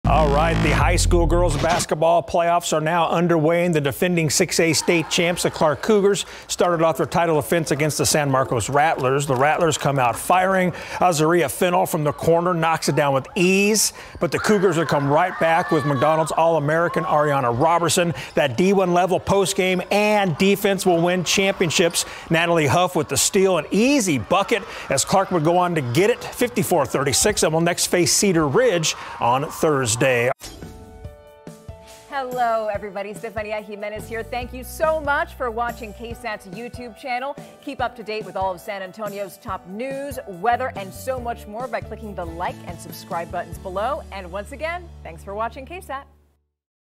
Defending state champions defeat San Marcos to begin girls’ basketball playoffs
The Texas High School Girls Basketball playoffs started up on Tuesday night. The defending 6-A champions, the Clark Cougars, began their title defense against the San Marcos Rattlers. The Cougars would go on to win this one 54-36.